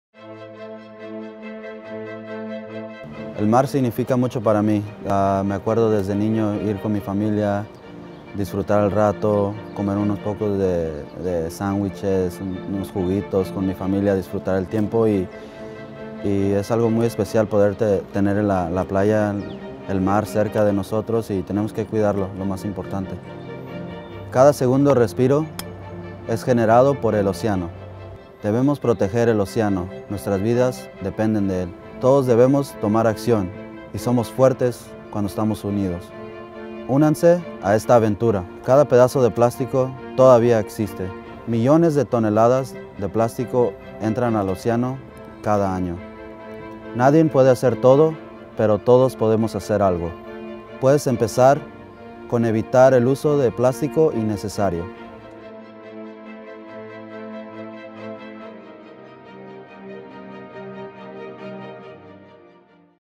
What protecting the ocean means to Jose Villarreal | Adidas - Parley
Find out how you can get your hands on the special LA Galaxy Parley Ocean Plastic jersey or stopping by the Foundation booth at the next Galaxy home game.